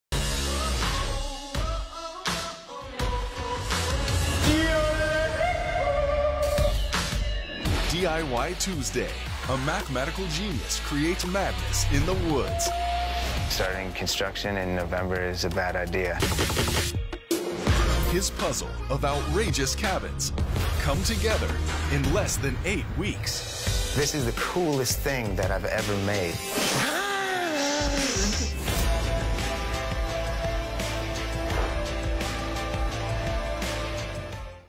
Wanderlust Cabin on DIY Network
A young cabin builder creates a one-of-a-kind cabin in upstate New York during the harsh winter months. Because of the new and complicated design, the crew must adapt to the environment, work efficiently and leave no room for mistakes. The end result is the ultimate off-the-grid cathedral in the Catskills.